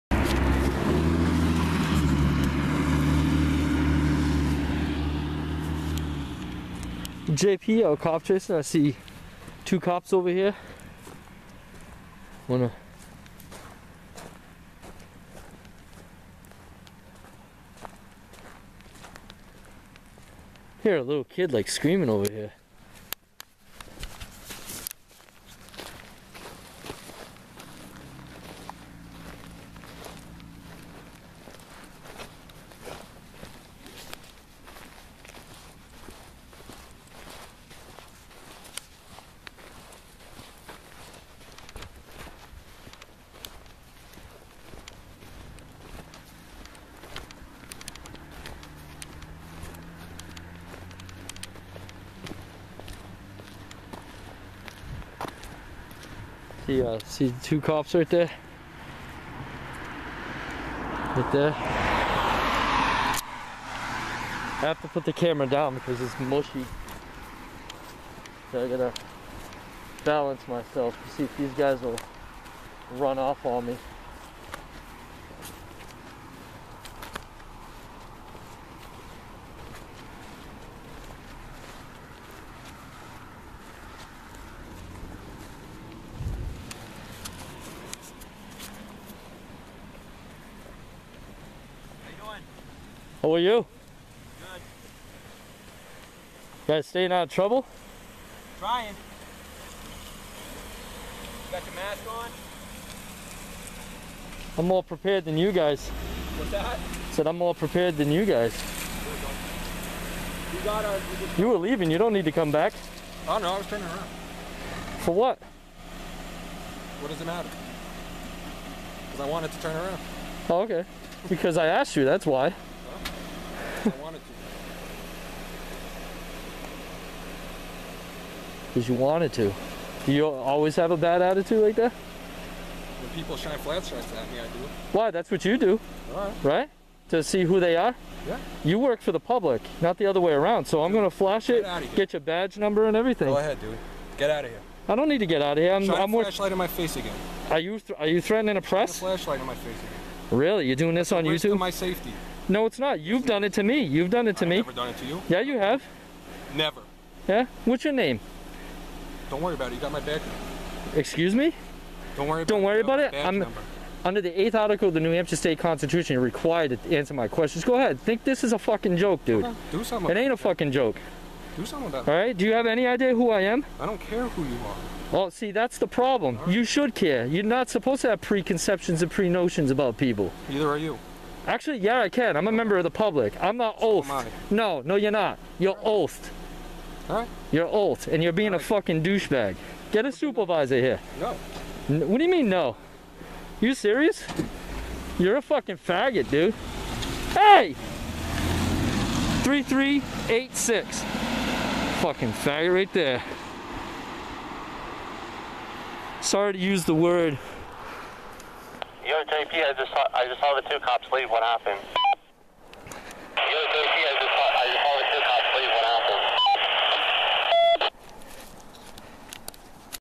Cop Accosted A Cop watcher
First time in a year JP Goes out then this cop meets one of the first and the best to do this ..